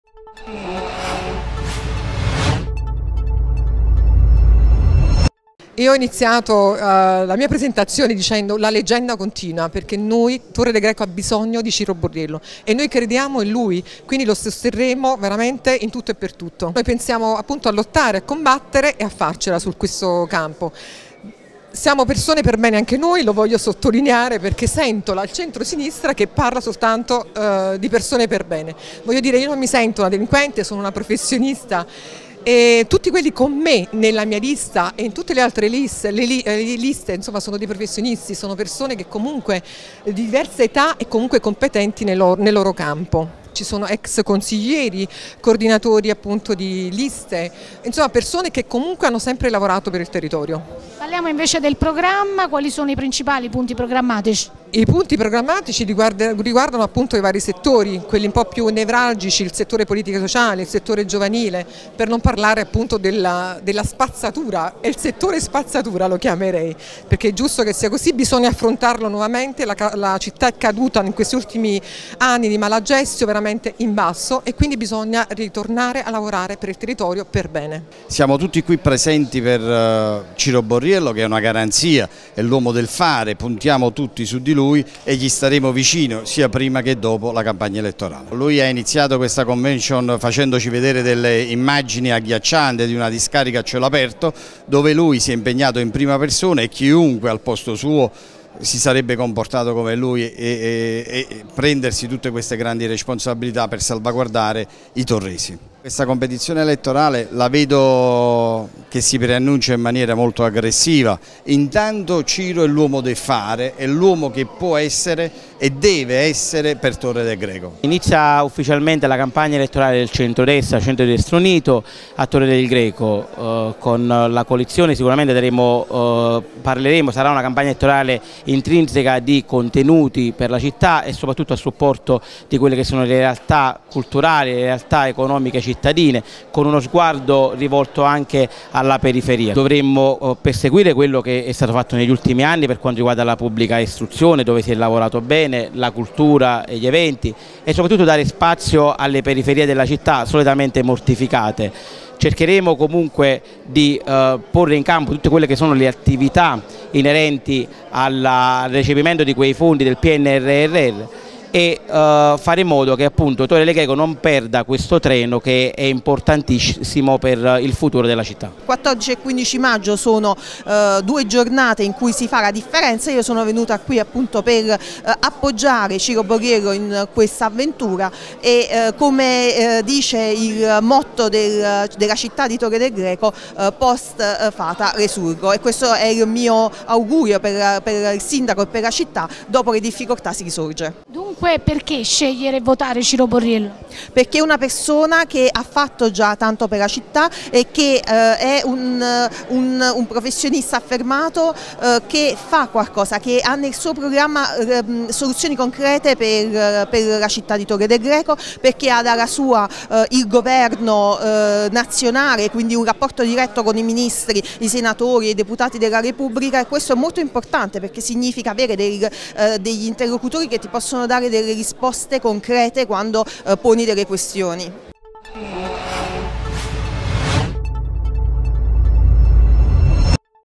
Forza Italia compatta per Ciro Borriello: “Ecco perché lo sosteniamo”
All’apertura della campagna elettorale del centrodestra, gli esponenti di Forza Italia senza tentennamenti: Ciro Borriello è il sindaco giusto per Torre del Greco.

Ai microfoni di TvCity, il senatore Francesco Silvestri, la deputata Isabella Adinolfi, e i referenti cittadini, Romina Stilo e Michele Langella.